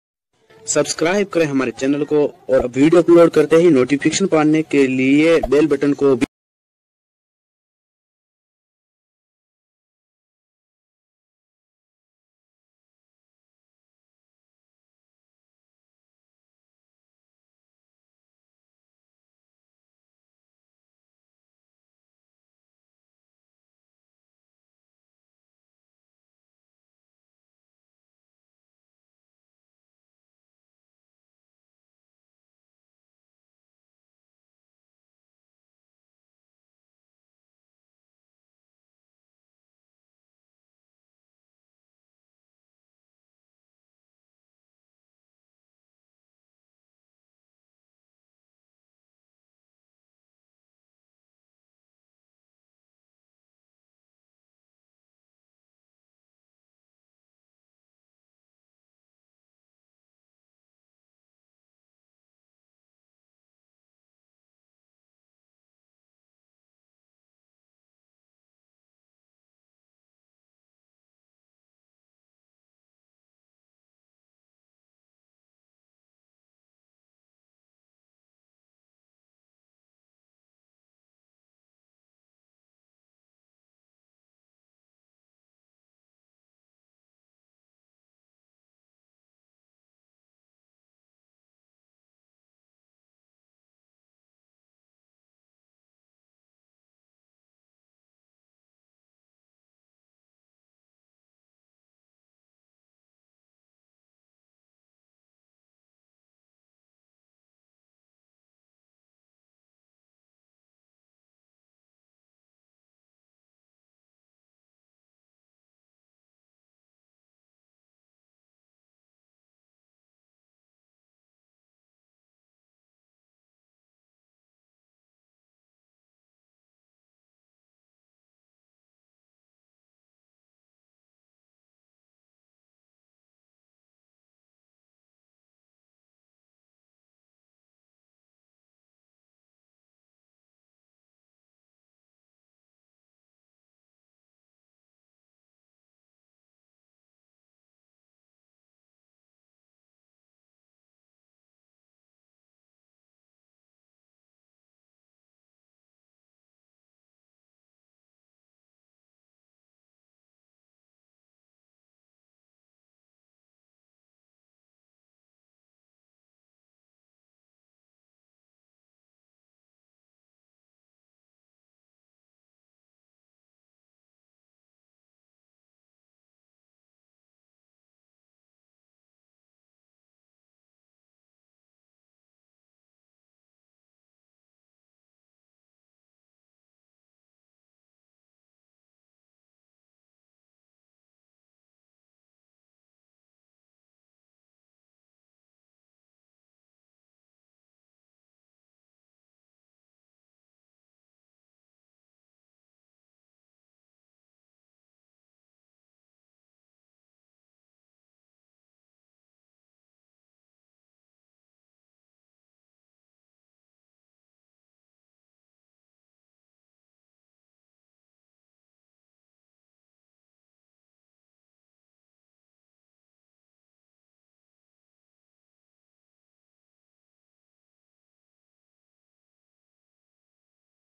There is a simple way to put the roof of the house मकान का छत डालते का ए है असान तरीका
There is a simple way to put the roof of the house
मकान का छत डालते का ए है असान तरीका
इस वीडियो मे आप देख सकते हैं कि छत का लिन्टर कैसे डालते है